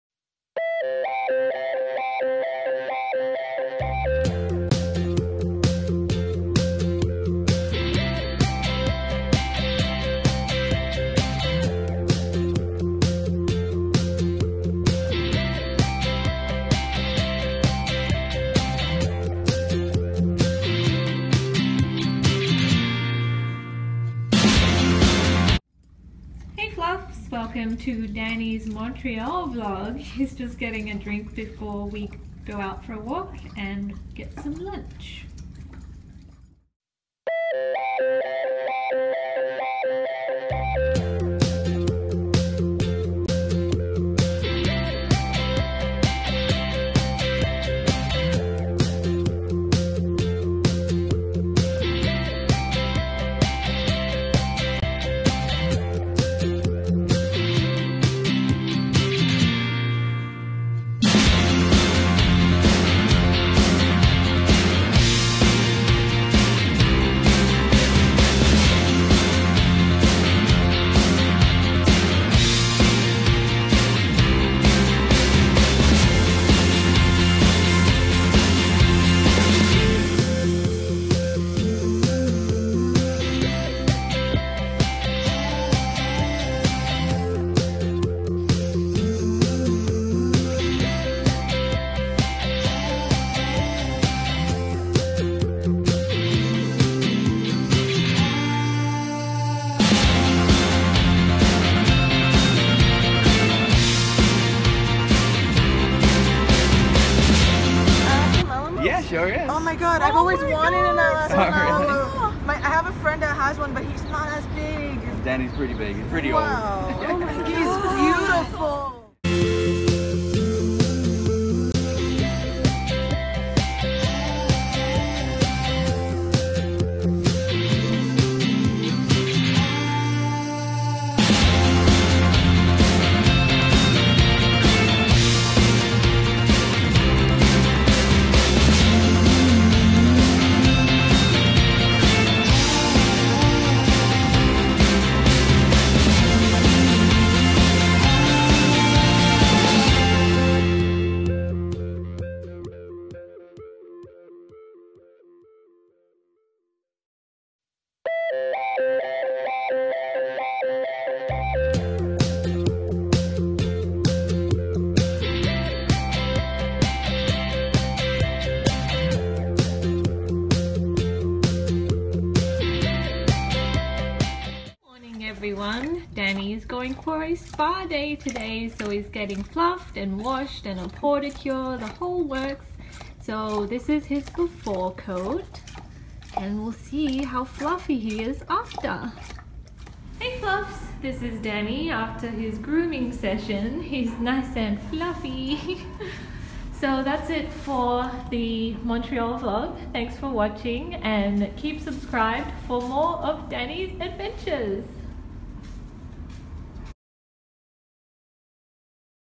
My Alaskan Malamute goes to Montréal!
Hi Fluffs!
As always there's no real fur on this channel except for Danny's fur!
Join Danny on his adventure to Montréal!!

And until next time, stay fluffy!
Danny and Raven